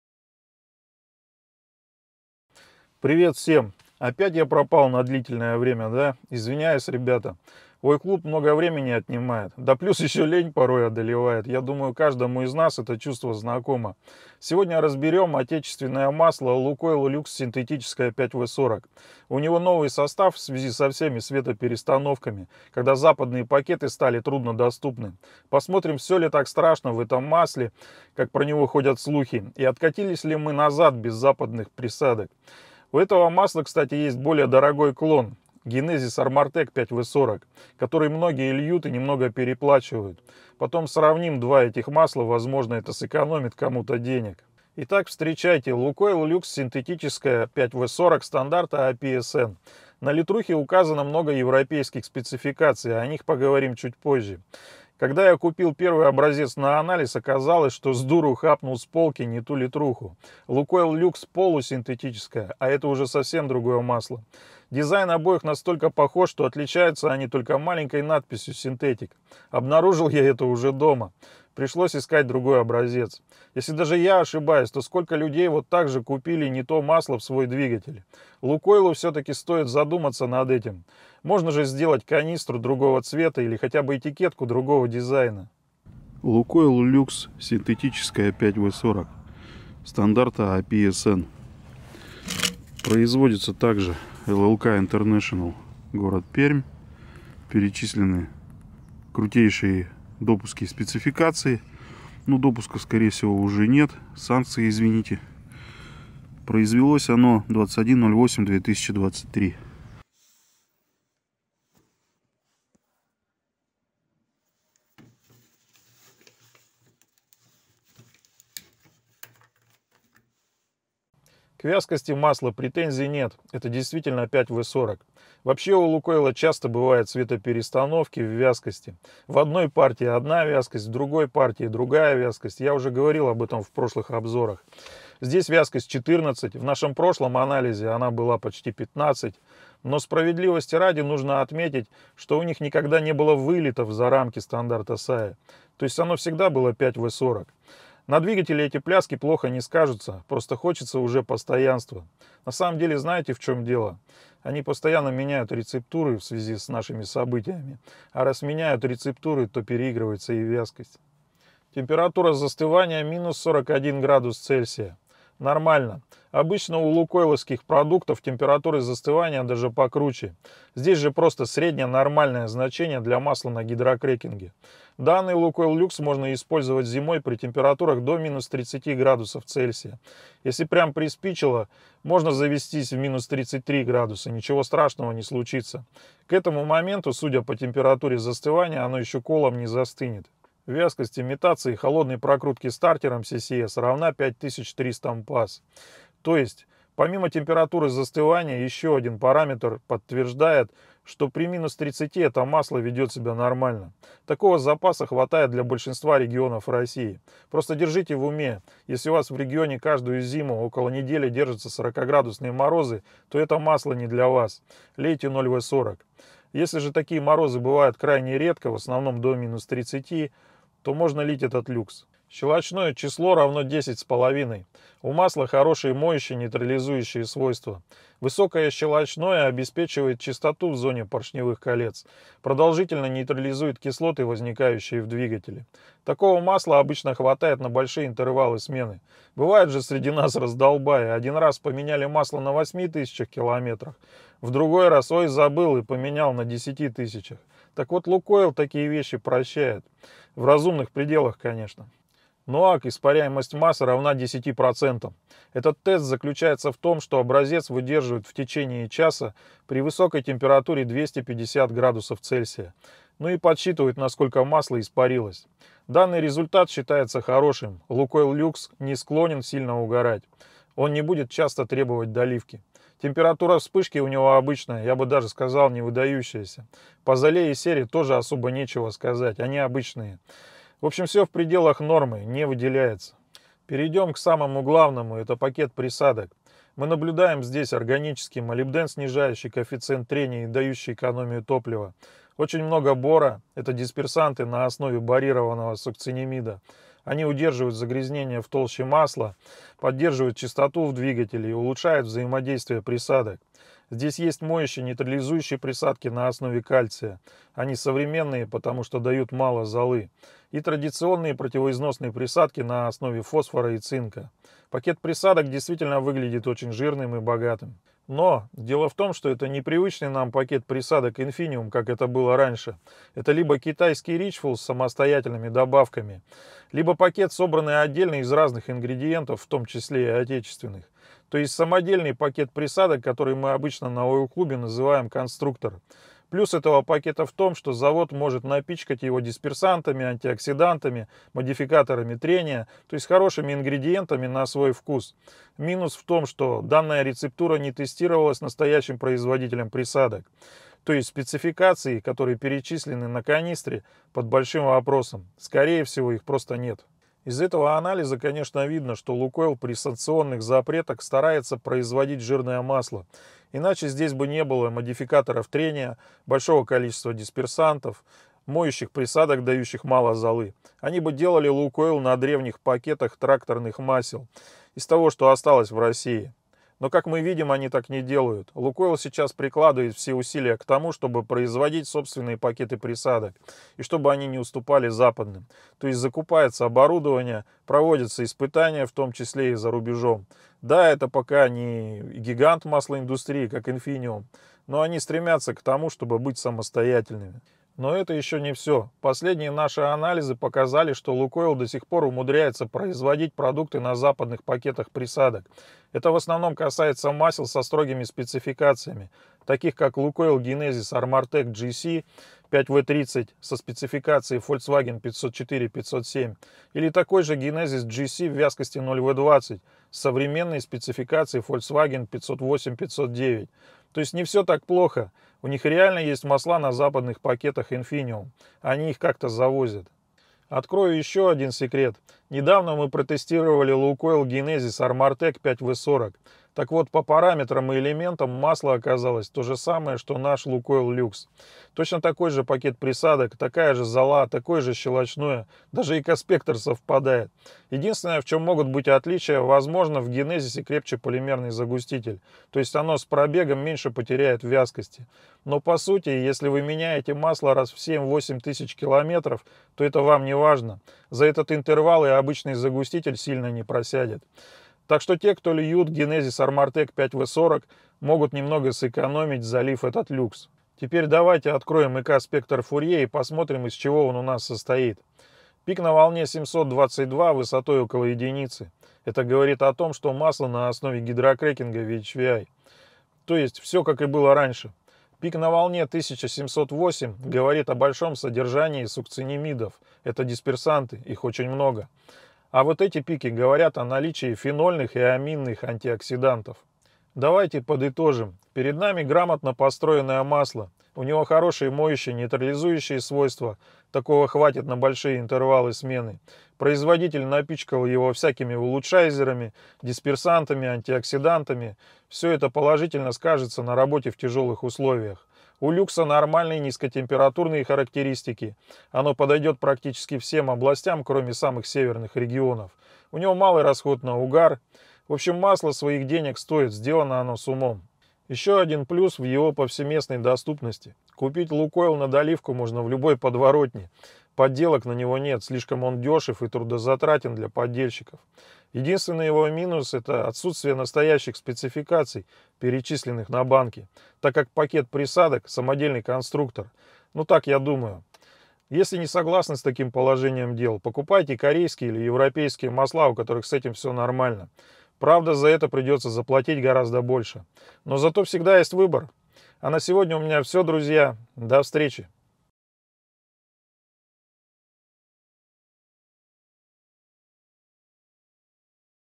НОВЫЙ Лукойл Люкс Синтетическое 5W-40 API SN - обзор на Ойл Клубе
Российское моторное масло Лукойл Люкс Синтетическое 5W-40 API SN.

Заявленные спецификации: API SN/CF; ACEA A3/B4; MB 226.5/229.3; VW 502 00/505 00;
Renault RN 0700/0710; Fiat 9.55535-N2; PSA B71 2296; АО "АВТОВАЗ".

Скидываемся на анализы масел тут (только поиском внизу форума проверяйте, не было ли анализа такого масла...)

Спасибо автолюбителям, которые скинулись на проведение лабораторного анализа!